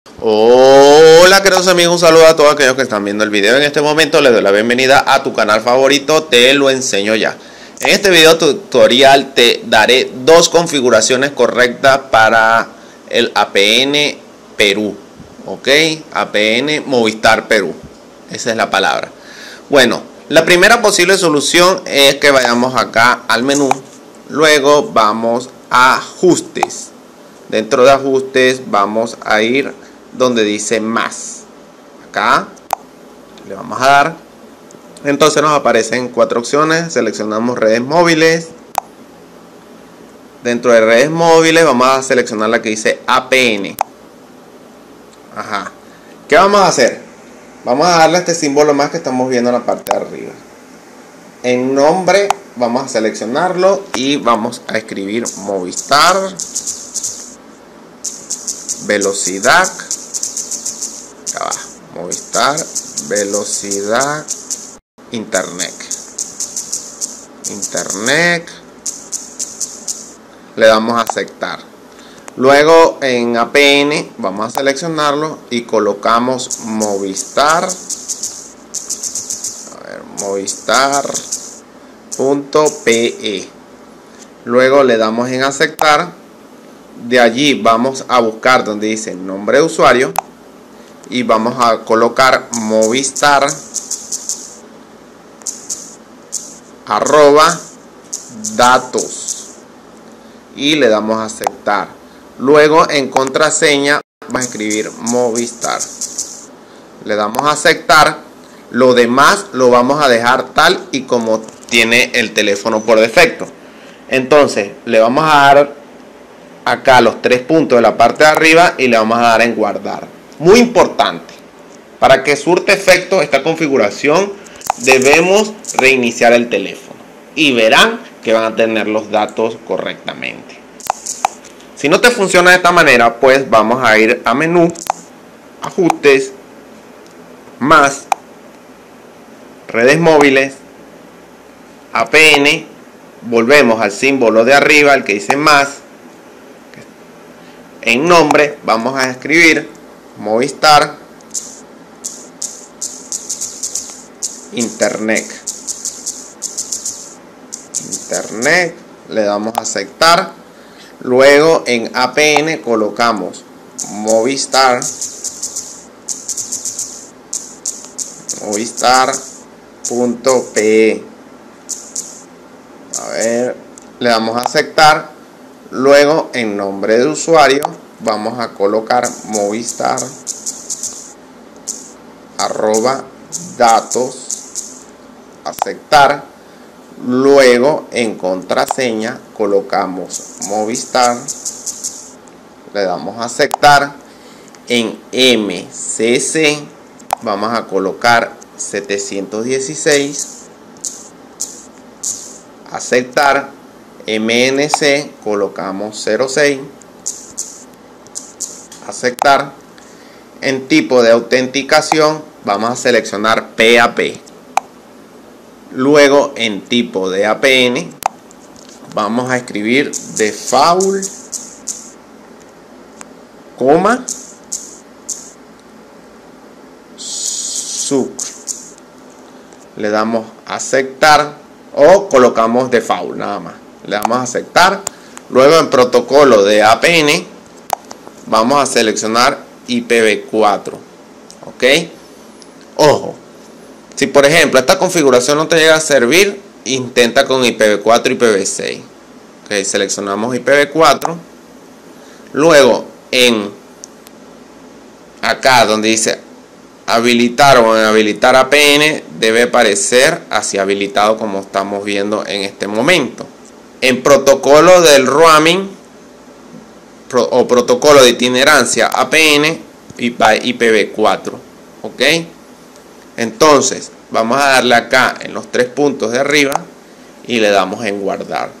Configurar APN Movistar Peru 3G/4G LTE I 2 Soluciones 2025 📶
En este vídeo tutorial te ayudare a Configurar APN Movistar Peru 3G/4G LTE de una manera fácil y sencilla por medio de 2 soluciones que cualquier persona puede realizar

Videos Recomendados
💛 Como ACELERAR Celular